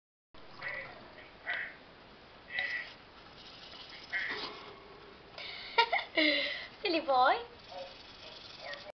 My Gerbil Clinging on the cage walls!
Nibbles wanted to come out and play so he was protesting by swinging on the cage walls! He's so crazy!